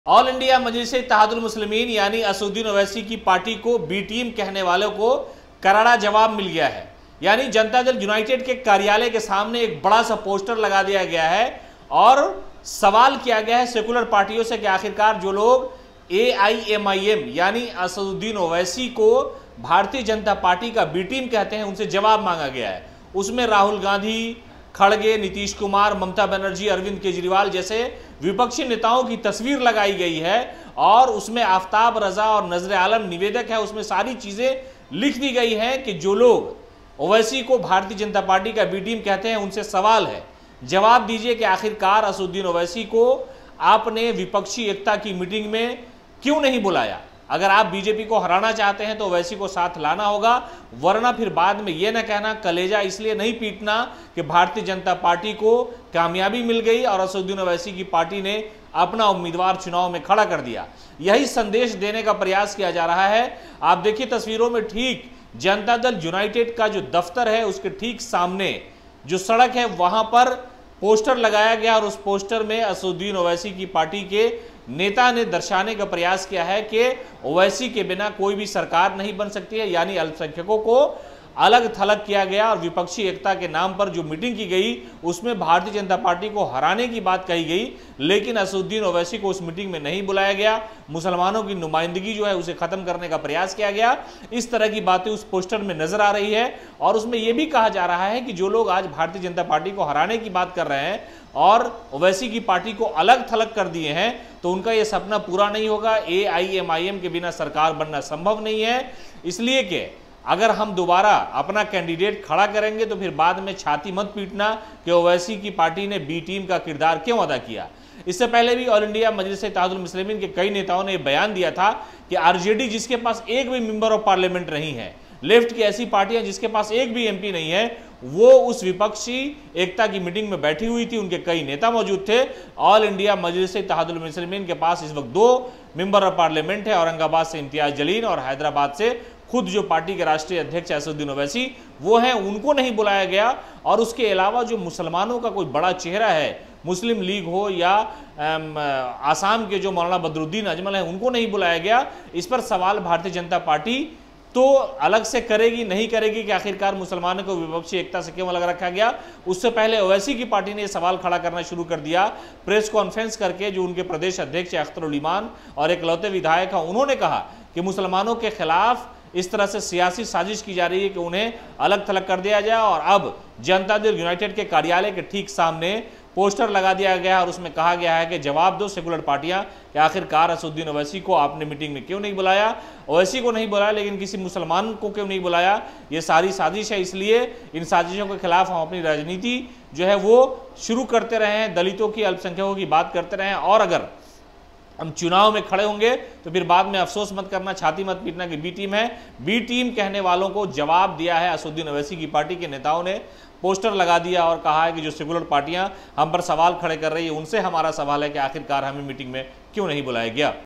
AIMIM ने JDU Office के बाहर चिपका दिया ऐसा पोस्टर की मच गया बवाल |Headlines Bihar
👉Related Search:
Bihar News
RJD
Tejashwi Yadav
Lalu Yadav
Nitish Kumar
Chirag Paswan